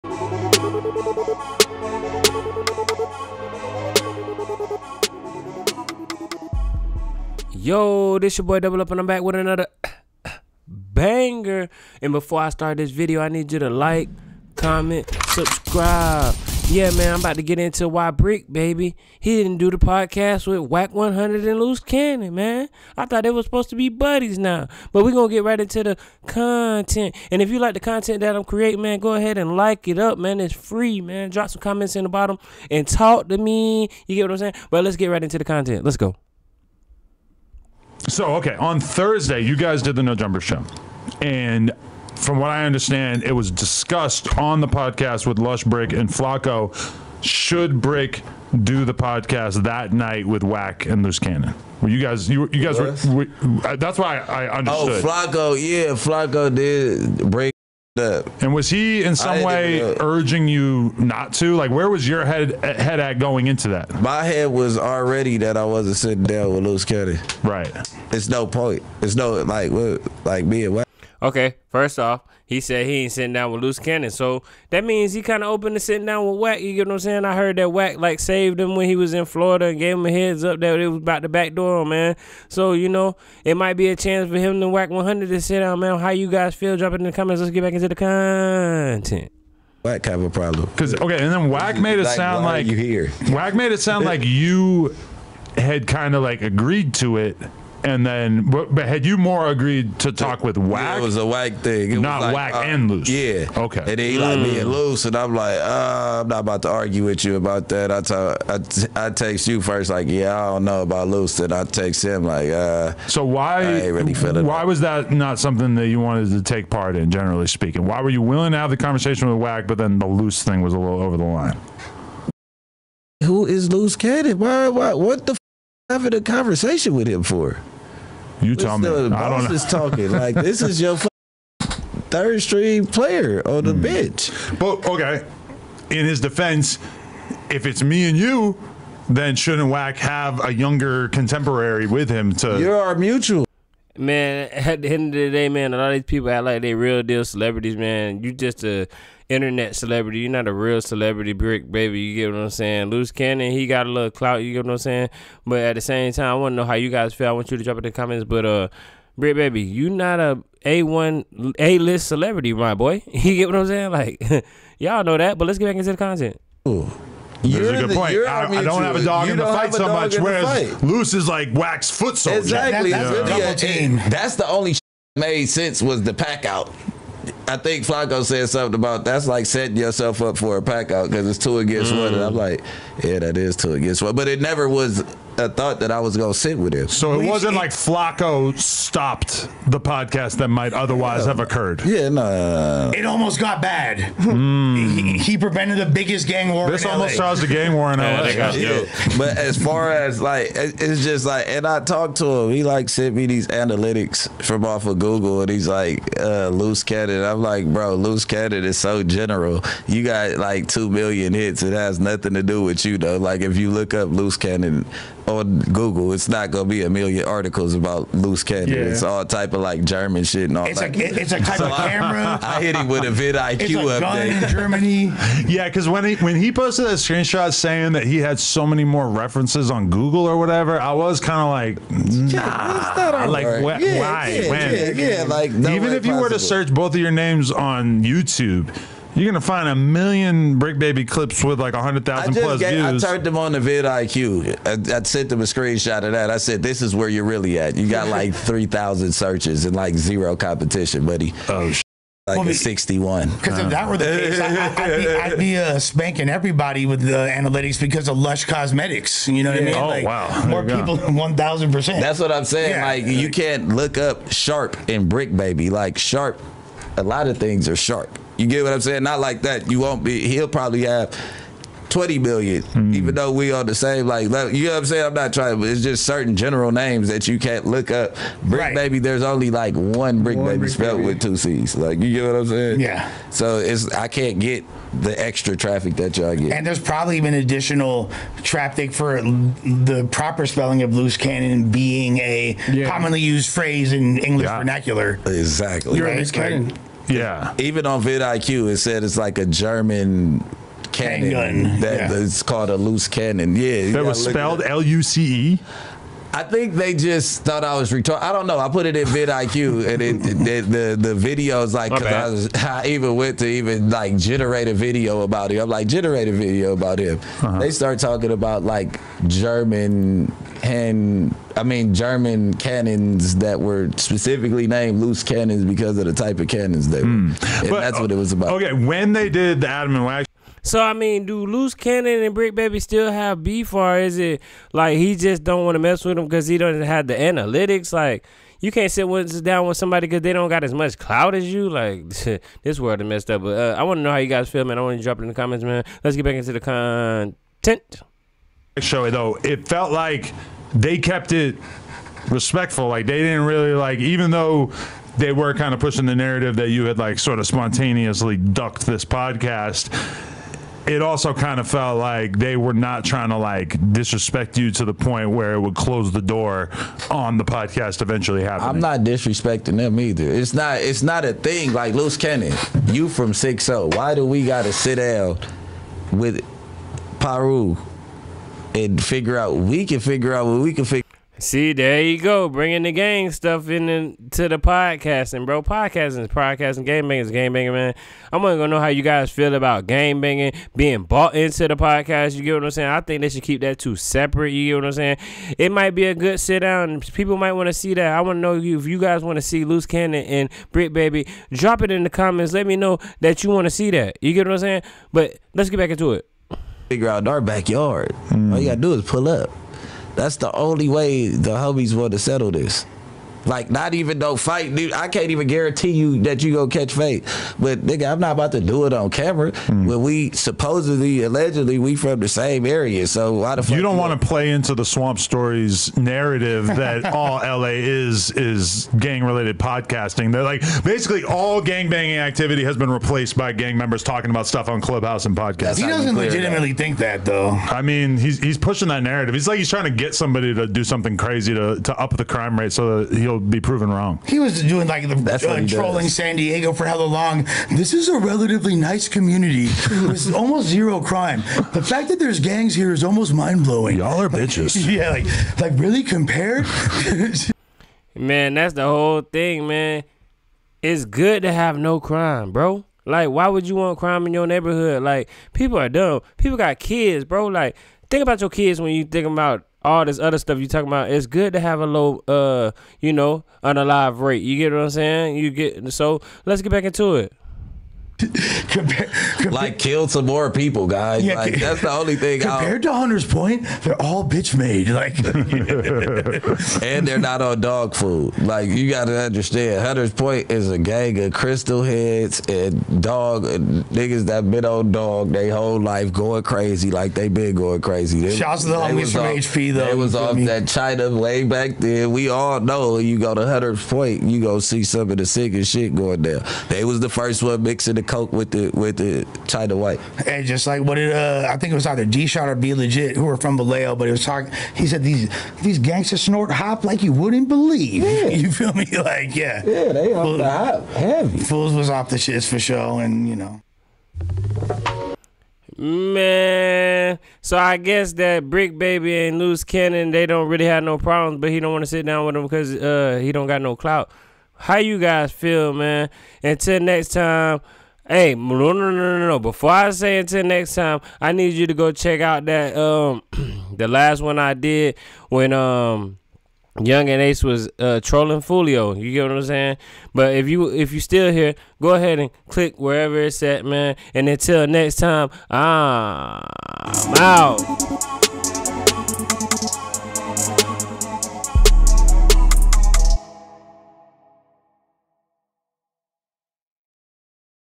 Why Bricc Baby Avoided Wack 100 & Luce Cannon on No Jumper
In this exclusive interview, Bricc Baby opens up about his decision to avoid a podcast session with Wack100 and Luce Cannon on the popular No Jumper platform. Discover the behind-the-scenes reasons that led to this intriguing choice, as Bricc Baby discusses the dynamics of the hip-hop industry, personal beliefs, and potential conflicts. Tune in for all the details and get a closer look at Bricc Baby's world.